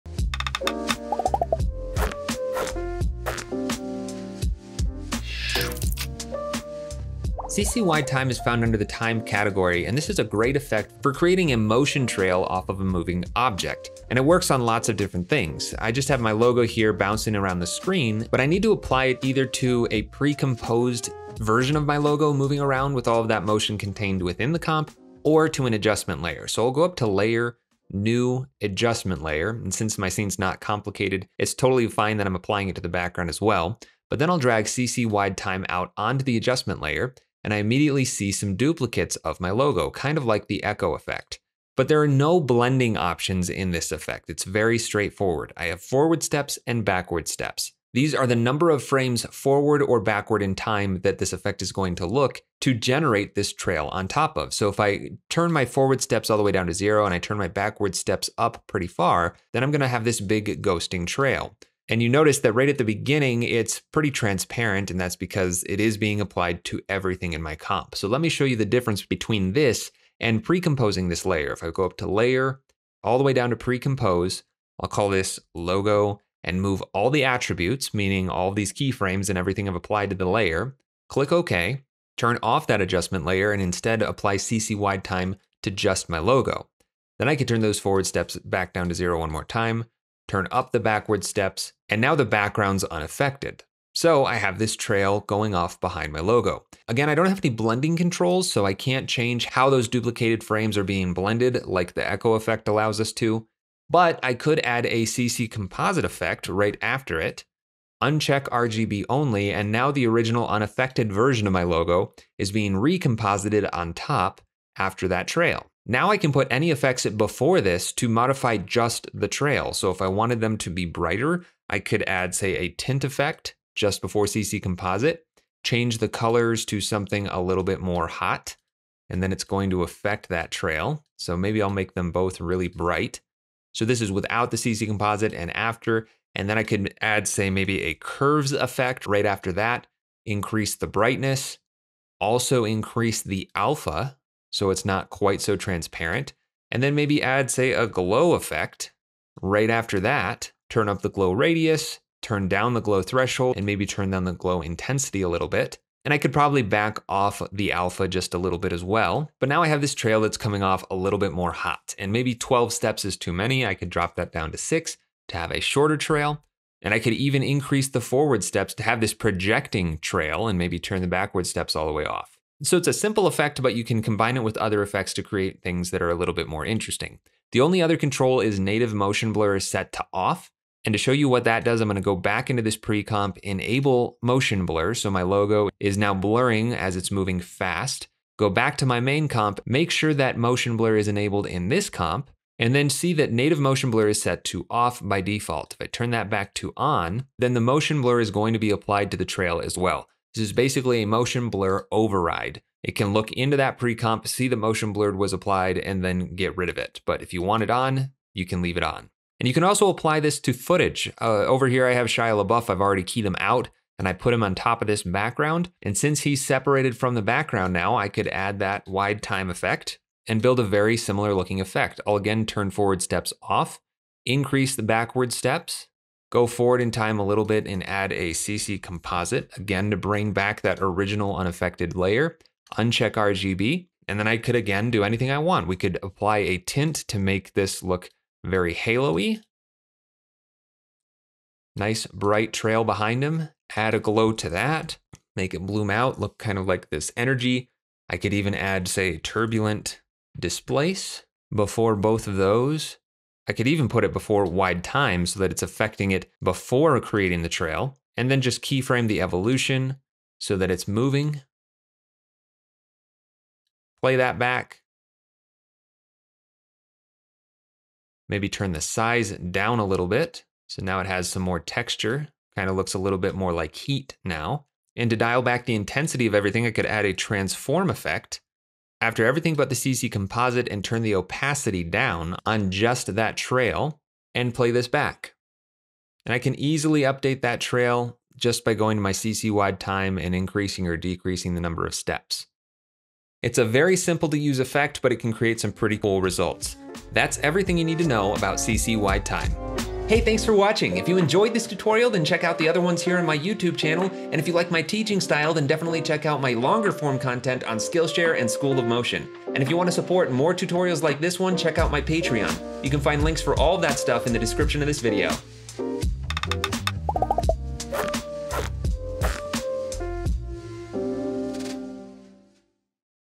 CC Wide Time | Effects of After Effects
In this quick tutorial, I explain how to use the CC Wide Time effect in Adobe After Effects.

Chapters:
00:00 Intro
00:07 CC Wide Time
04:36 Thanks for watching!